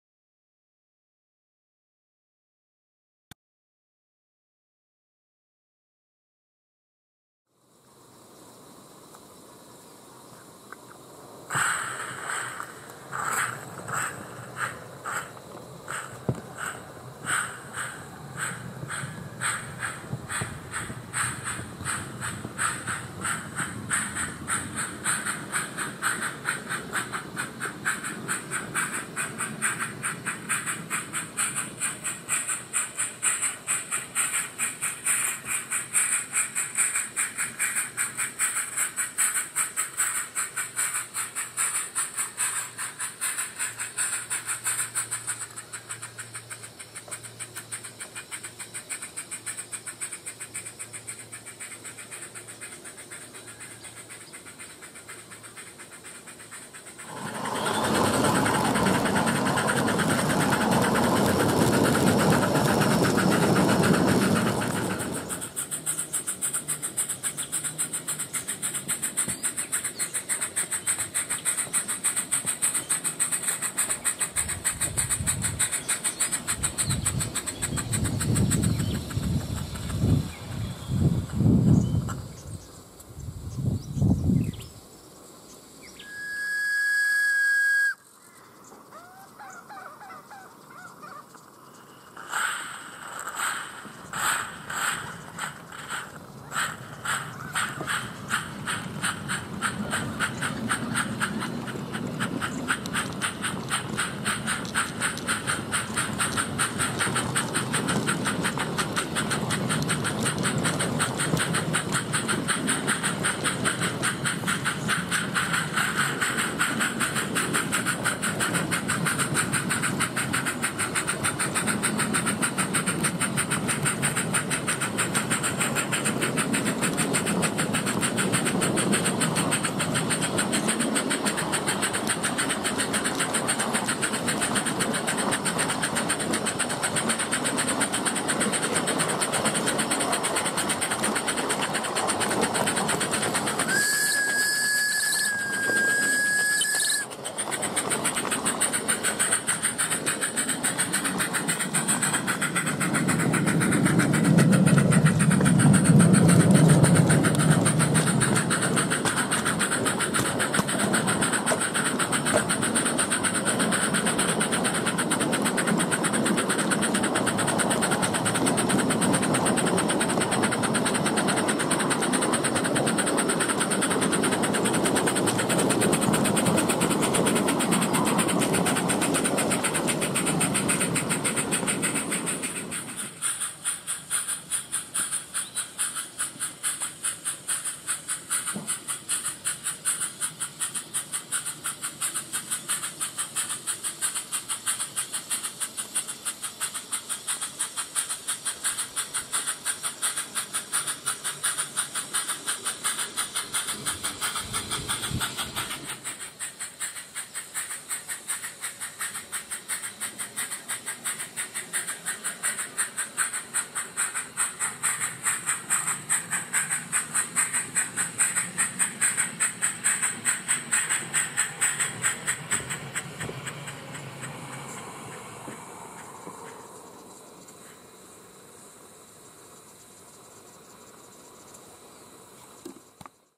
Hunslet Waril Class No10
Hunslet Waril Class No10 Working on the Brimhillah Light Railway. Fitted with Summerlands Chuffer and SSP Slomo.